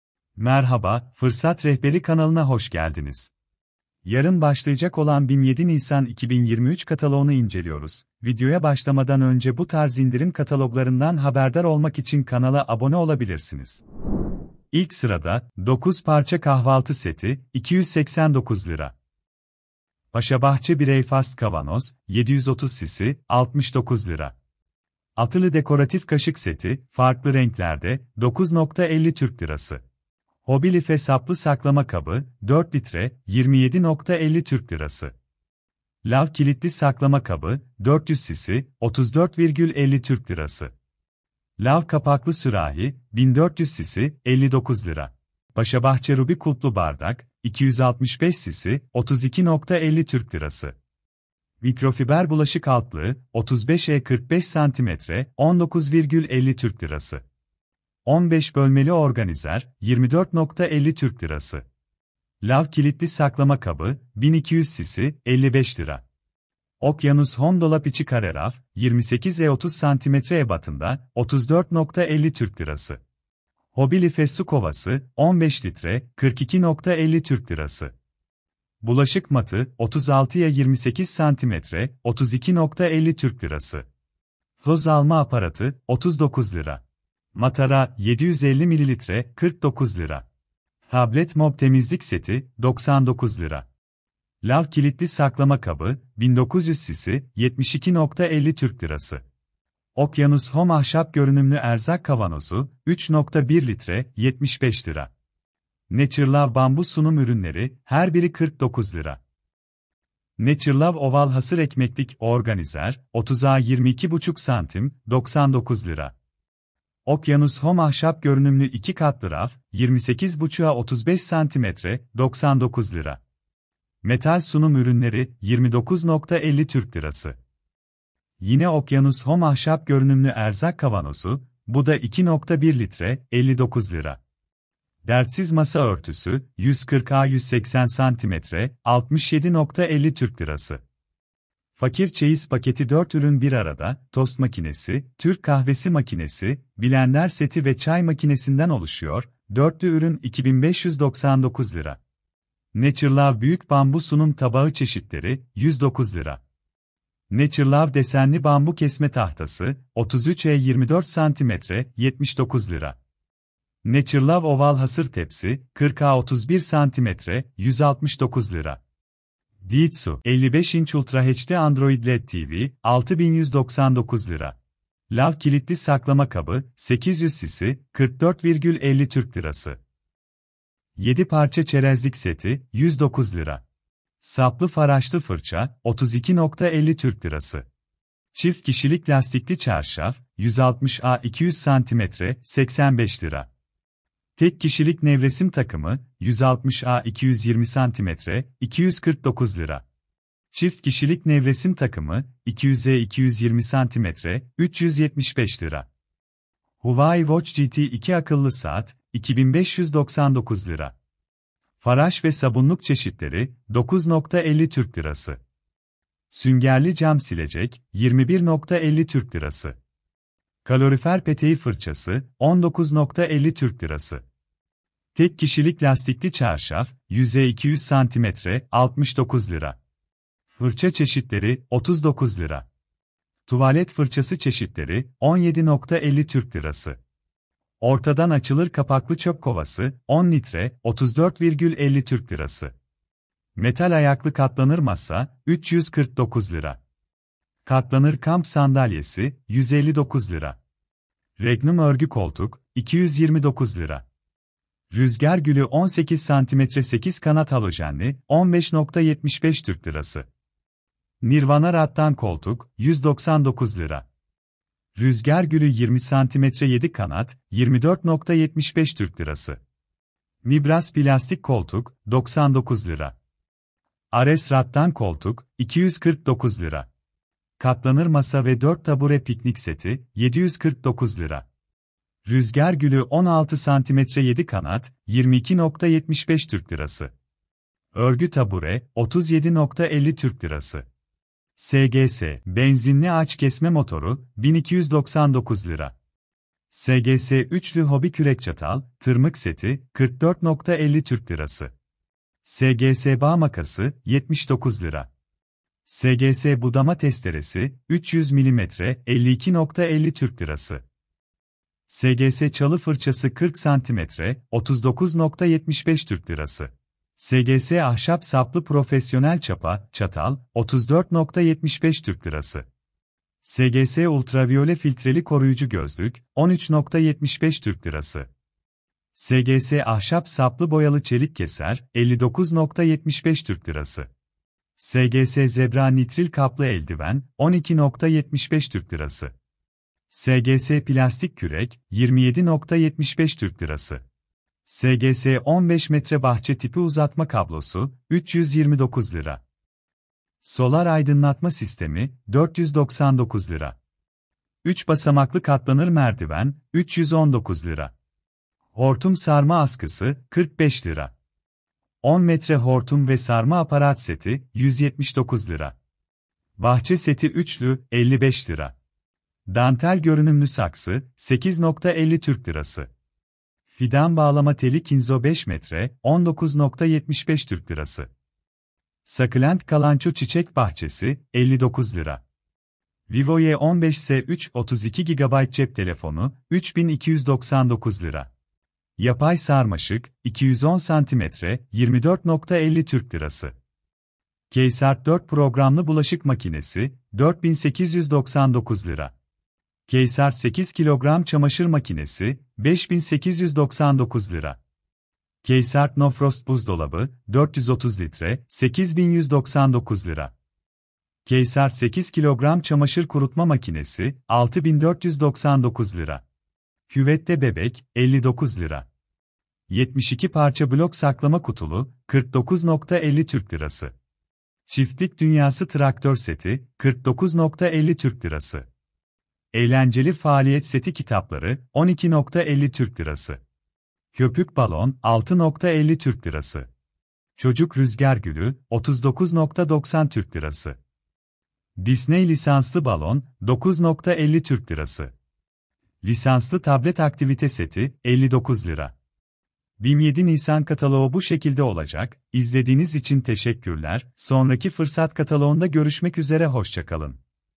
✅BİM BU CUMA✅BİM 7 NİSAN 2023 KATALOĞU | BİM AKTÜEL ÇEYİZLİKLER | HIRDAVAT | BEYAZ EŞYA BİM İNDİRİM
Yarın başlayacak olan bim 7 nisan 2023 kataloğunun detaylarını inceliyoruz. Bu hafta bim marketlerinde ne var sorusunun cevabı kanalımızda. Çeyizlik ürünler, beyaz eşya ve hırdavat ürünleri bu hafta bim market aktüel katalğunda yer alacak. Fırsat Rehberi kanalımızda net görsellerle tek tek inceliyoruz.

Naturalove Desenli Bambu Kesme Tahtası ~ 33x24 cm
Naturalove Büyük Bambu Sunum Tabağı Çeşitleri ~ 30x30 cm
Naturalove Bambu Sunum Ürünleri
Çerezlik Seti
Kahvaltı Seti
Naturalove Oval Hasır Ekmeklik/organizer ~ 30x22.5 cm
Naturalove Oval Hasır Tepsi ~ 40x31 cm
Tablet Mob Temizlik Seti
Microfiber Bulaşık Altlığı 35x45 cm Yıkanabilir
Bulaşık Matı ~36x28 cm
SGS Ahşap Saplı Profesyonel Çapa/çatal
SGS 3’lü Hobi Kürek-çatal -tırmık Seti
SGS Zebra Nitril Kaplı Eldiven
Dertsiz Masa Örtüsü 140x180 cm Dilimli Kenar
Metal Sunum Ürünleri
Okyanus Home Ahşap Görünümlü Erzak Kavanozu ~ 2.1 L
Okyanus Home Ahşap Görünümlü Erzak Kavanozu ~ 3.1 L
Okyanus Home Ahşap Görünümlü İki Katlı Raf ~ 28.5x35 cm
Fakir Çeyiz Paketi 4 Ürün 1 Arada
Metal Ayaklı Katlanır Masa
Katlanır Masa ve 4 Tabure Piknik Seti
Dekoratif Kaşık Seti Farklı Renklerde
Okyanus Home Dolap İçi Kare Raf ~28x30 cm
Matara ~750 Ml
15 Bölmeli Organizer Farklı Renklerde
Toz Alma Aparatı
Keysmart Key 80 Krt 8 Kg Çamaşır Kurutma Makinesi
Keysmart Key 4014 Bm 4 Programlı Bulaşık Makinesi
Vivo Y15s 3/32 GB Ekran: 6.51 İnç Ram: 3 GB
Lav Kapaklı Sürahi ~1400 cc
Lav Kilitli Saklama Kabı ~1900 cc
Ortadan Açılır Kapaklı Çöp Kovası ~10 L
Nevresim Takımı Çift Kişilik Nevresim: 200x220 cm Çarşaf: 240x260 cm
Nevresim Takımı Tek Kişilik Nevresim: 160x220 cm Çarşaf: 160x240 cm
Lav Kilitli Saklama Kabı ~1200 cc
Lav Kilitli Saklama Kabı ~800 cc
Lav Kilitli Saklama Kabı ~400 cc
Hobby Life Saplı Saklama Kabı ~4 L
Hobby Life Su Kovası ~15 L
Paşabahçe Breakfast Kavanoz ~730 cc
Paşabahçe Ruby Kulplu Bardak ~265 cc
Faraş Ve Sabunluk Çeşitleri
Süngerli Cam Silecek Farklı Renklerde
Kalorifer Peteği Fırçası
Fırça Çeşitleri
Saplı Faraşlı Fırça
Tuvalet Fırçası Çeşitleri
Çift Kişilik Lastikli Çarşaf 160x200 cm +25 cm ± %5 cm
Tek Kişilik Lastikli Çarşaf 100x200 cm +25 cm ± %5 cm
Keysmart Key 430 Bznf  No-Frost Buzdolabı 430 L
Keysmart Key 8002 cm 8 Kg Çamaşır Makinesi
Nirvana Rattan Koltuk
Nibras Plastik Koltuk
Katlanır Kamp Sandalyesi
Huawei Watch Gt 2 Akıllı Saat
Dijitsu 55 " Uhd Android Led Tv
Ares Rattan Koltuk
Regnum Örgü Koltuk
Örgü Tabure
SGS Bağ Makası
SGS Budama Testeresi 300 Mm
SGS Plastik Kürek
SGS Çalı Fırçası 40 cm
3 Basamaklı Katlanır Merdiven
Hortum Sarma Askısı
10 M Hortum ve Sarma Aparat Seti
Yapay Sarmaşık ~210 cm Farklı Çeşitlerde
Fidan Bağlama Teli Kinzo 5 M
Dantel Görünümlü Saksı
Succulent Kalanchoe Çiçek Bahçesi
Bahçe Seti 3’lü
SGS Benzinli Ağaç Kesme Motoru
SGS Uv Filtreli Koruyucu Gözlük
SGS 15 m Bahçe Tipi Uzatma Kablosu 3x1x15 Metre
SGS Ahşap Saplı Boyalı Çelik Keser
Solar Aydınlatma Sistemi 9v 3.5w Solar Panel Dahili 6 Smd Led Fener
72 Parça Blok Saklama Kutulu
Eğlenceli Faaliyet Seti Kitapları
Çiftlik Dünyası Traktör Seti Farklı Çeşitlerde
Köpük Balon
Disney Lisanslı Balon Farklı Çeşitlerde